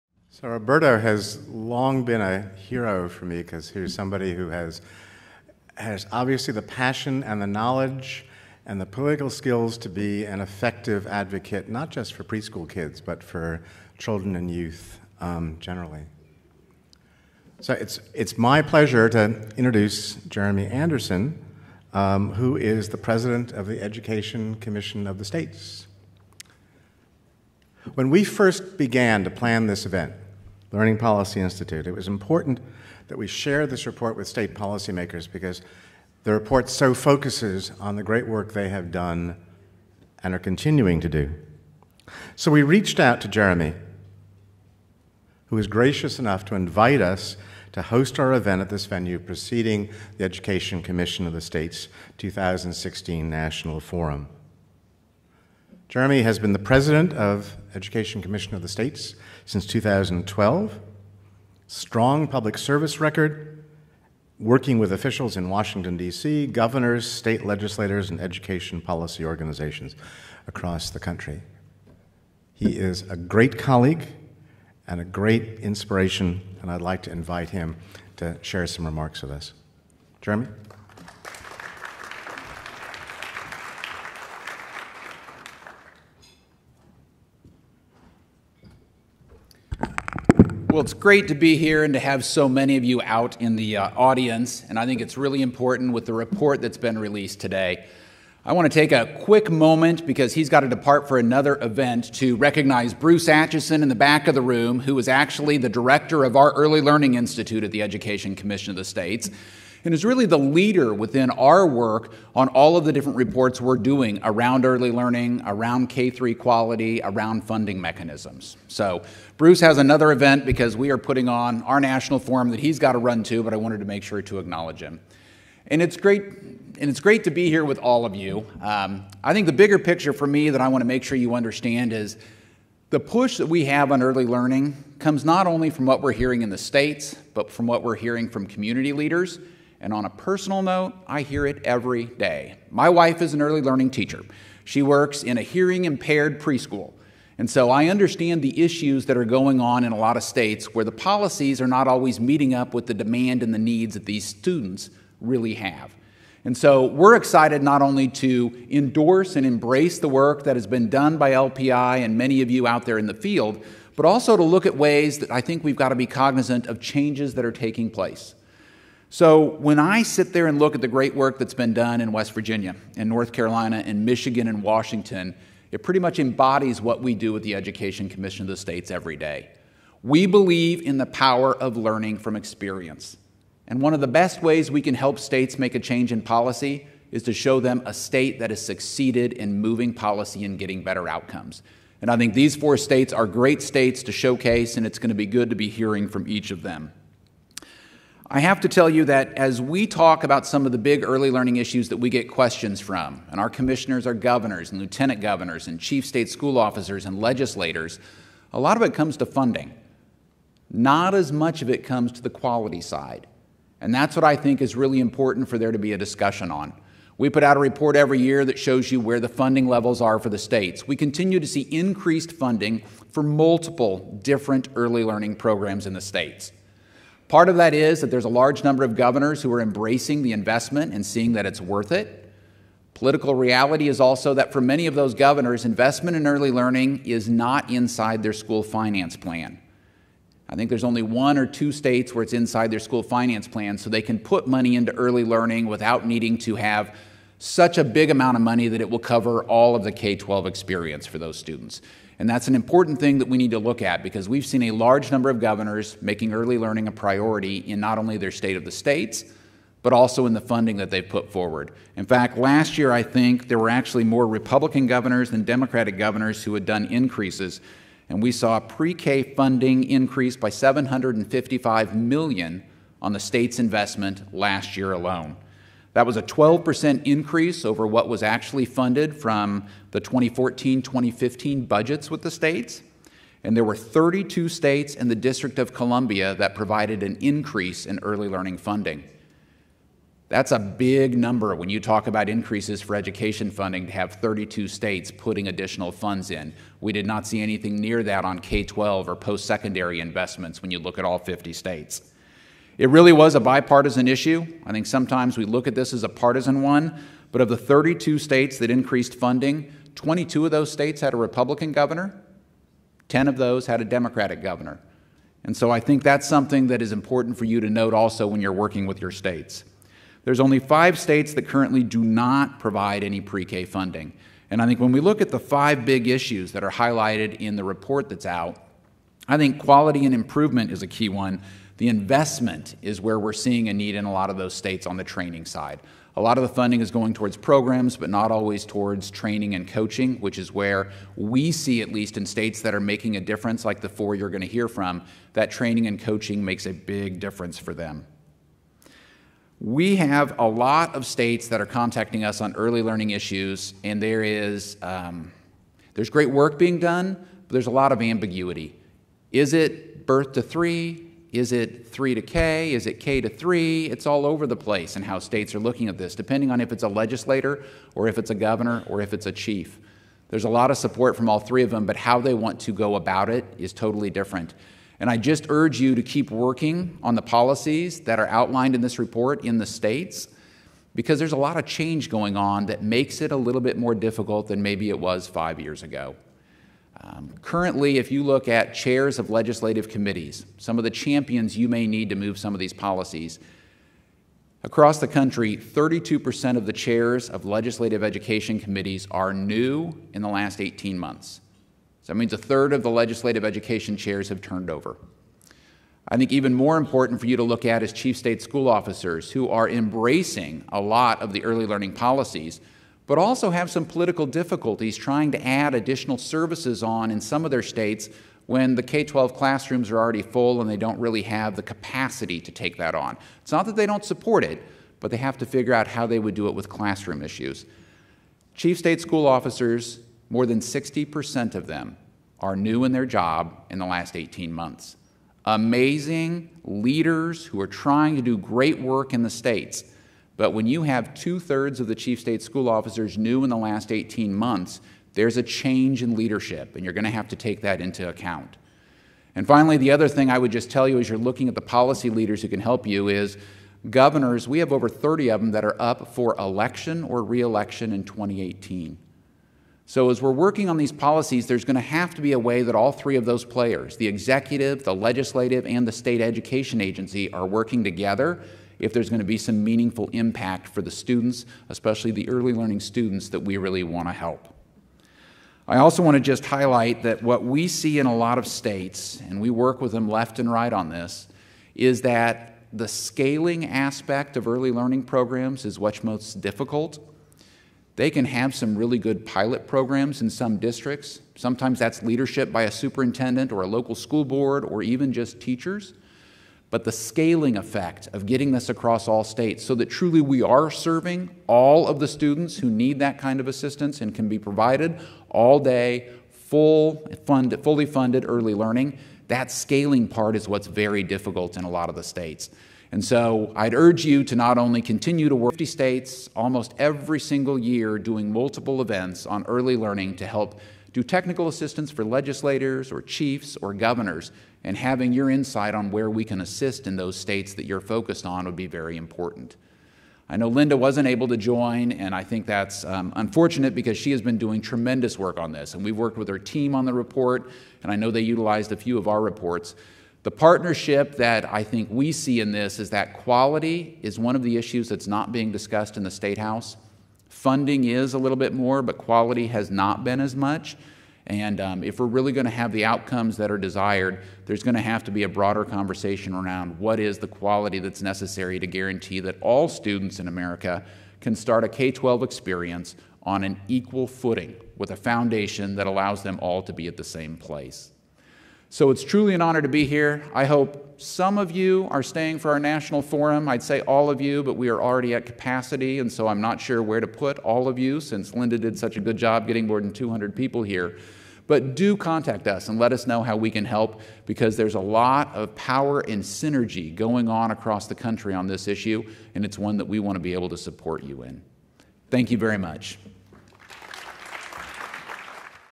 Jeremy Anderson - Road To High Quality Early Learning: Lessons from the States
The Learning Policy Institute, the Council for a Strong America, and the First Five Years Fund hosted the forum, The Road to High-Quality Early Learning: Lessons from the States on June 29, 2016 in Washington, D.C.

The event coincided with LPI’s release of The Road to High-Quality Early Learning: Lessons from the States, an in-depth review and analysis of four states — Michigan, North Carolina, Washington, and West Virginia — that have devised a set of strategies for delivering quality programs at scale.

The event featured a keynote address by Roberto Rodríguez, Deputy Assistant to the President for Education and The White House Domestic Policy Council and included remarks from representatives of partner organizations, including: Jeremy Anderson, Education Commission of the States, Kris Perry, the First Five Years Fund, and David Kass, Council for a Strong America.

Report lead author Marjorie Wechsler, Principal Research Manager at LPI, presented key findings, followed by a panel discussion with representatives from the four featured states.

The views, thoughts and opinions expressed in this video belong solely to the speakers, and do not necessarily reflect the perspective of the Learning Policy Institute.